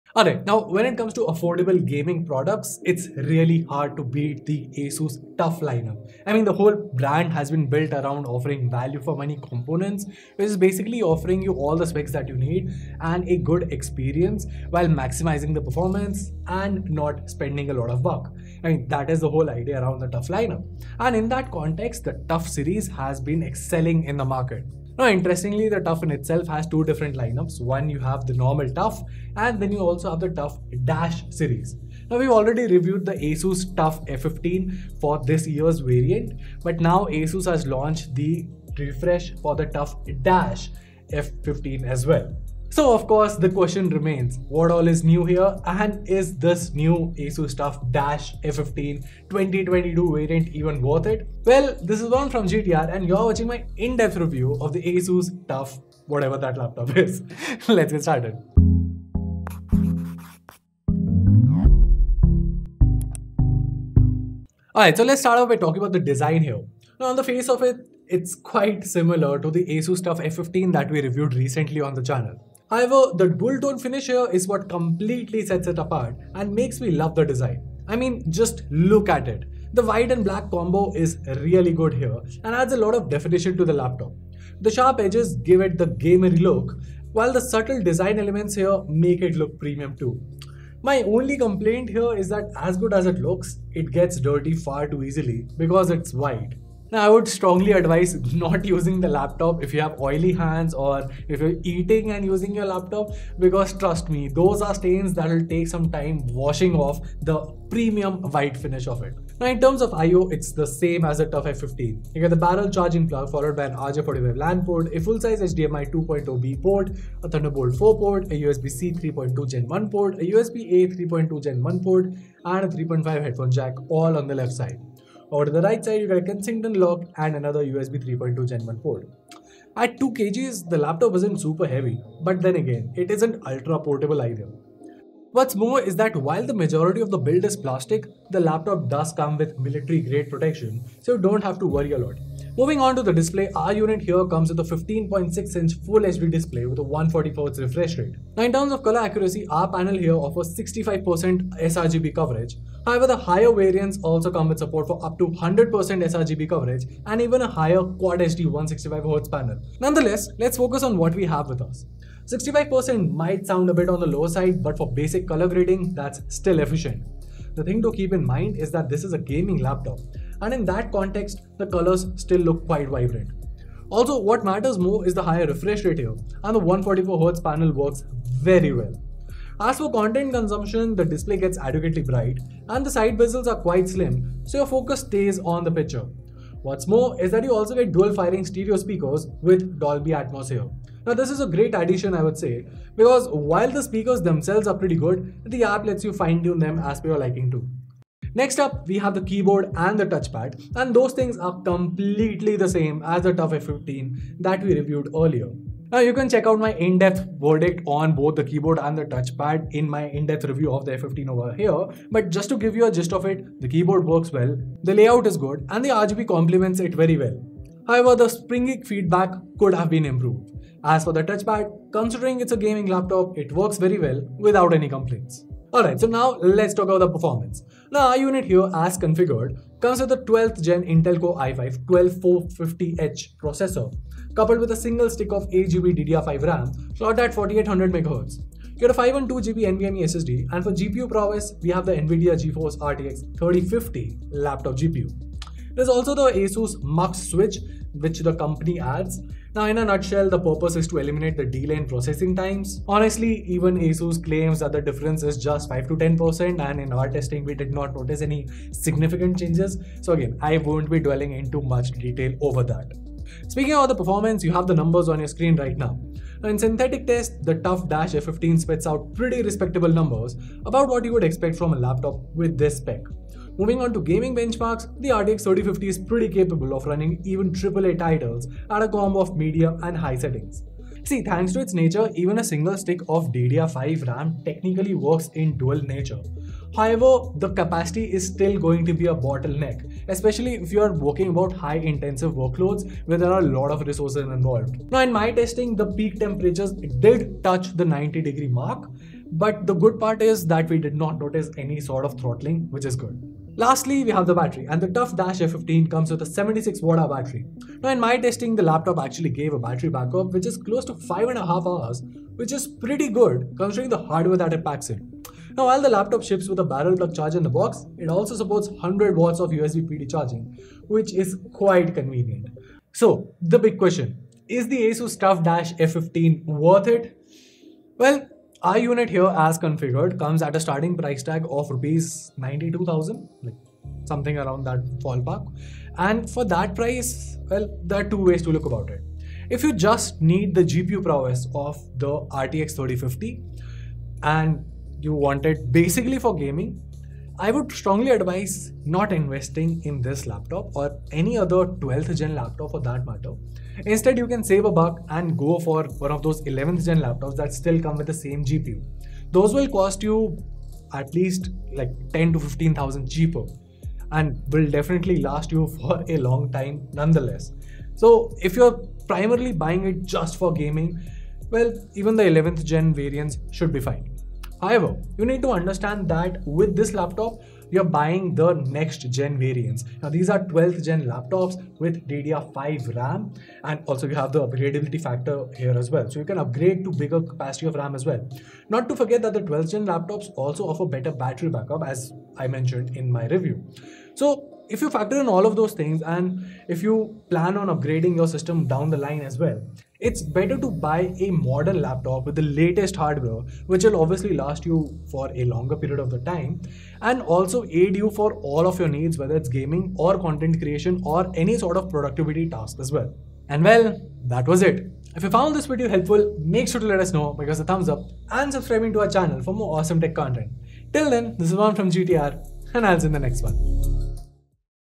ASUS TUF Dash F15 (2022) Laptop Review: Do You Need ROG Anymore? | GTR
Is the ASUS TUF Dash F15 (2022) the best slim gaming laptop out there? 🤔🤔

In today's video, we'll be taking a look at the ASUS TUF Dash F15 (2022) Gaming Laptop. This thing comes equipped with up to a Intel Core i7-12450H processor with newest DDR5 RAM alongside an Nvidia RTX 3050 GPU, and much more.

But is the ASUS TUF Dash F15 2022 Gaming Laptop worth the price? And should you pick it over the ASUS powered ASUS TUF Gaming A15 Laptop? Let's find out in our in-depth laptop review.

2022 tuf gaming
asus tuf gaming laptop review